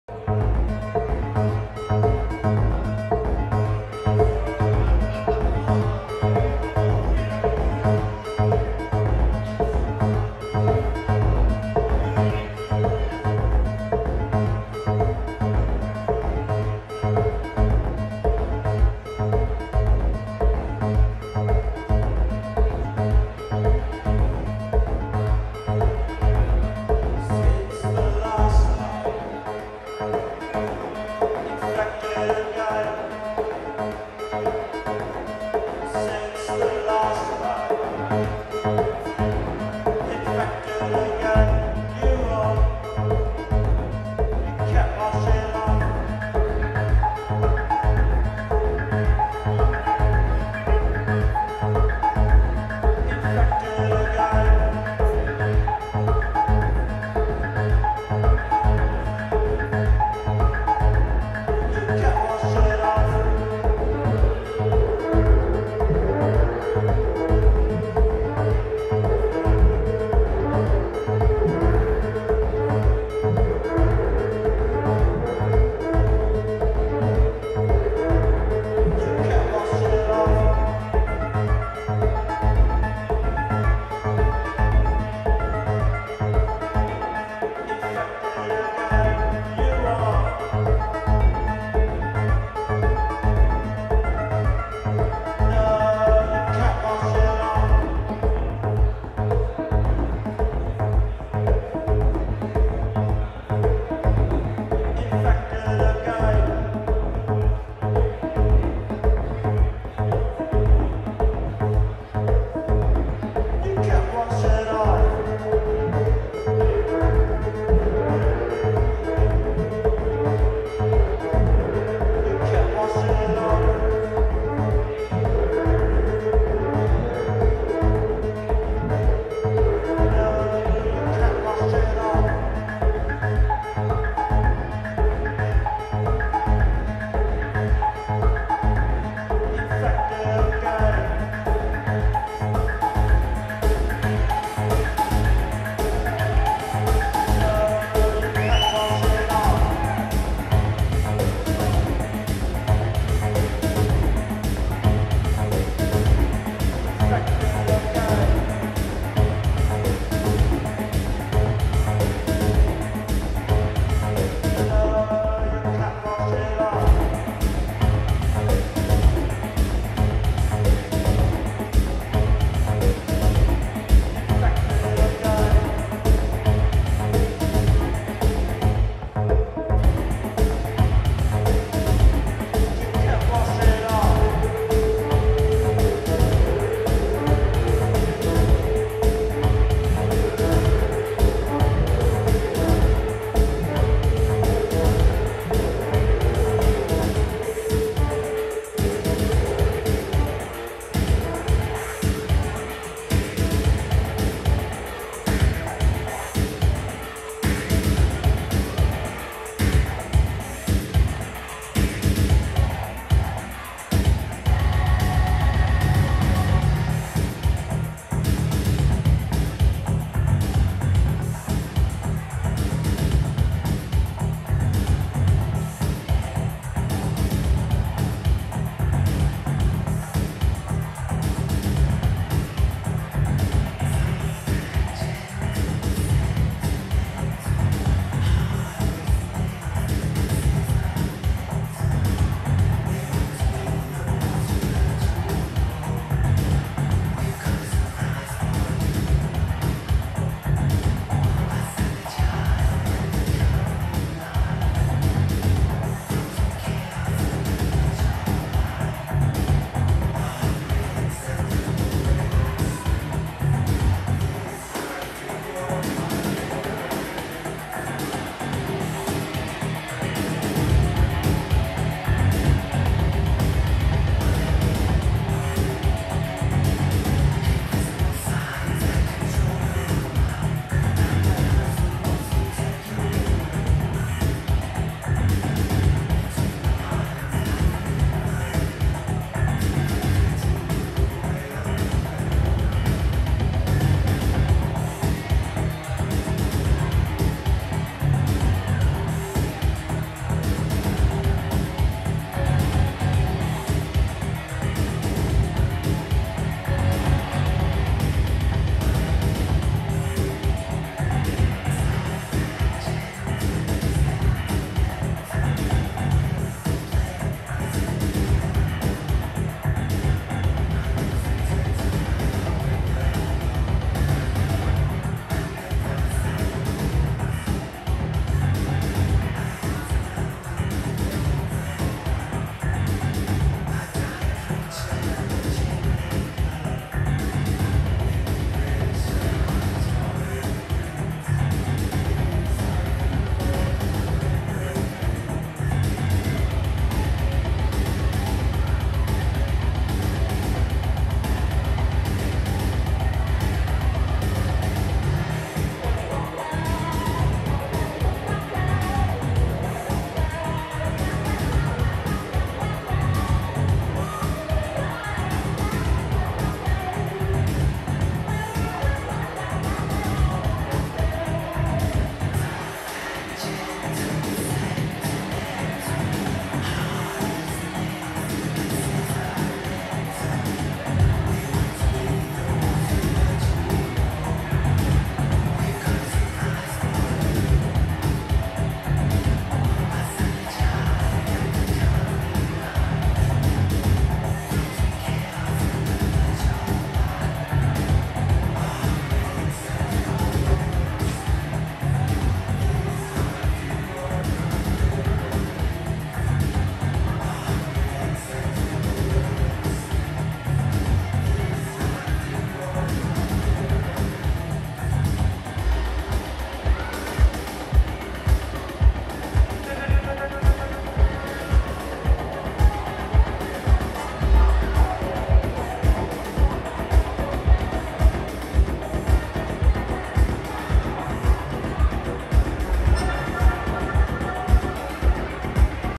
Job Sifre at DGTL Festival, Amsterdam 31 March 2018
Job Sifre at DGTL Festival, Amsterdam 31 March 2018. The video is recorded with an iPhone 6s with a Zoom iQ5 stereo microphone. Support the artist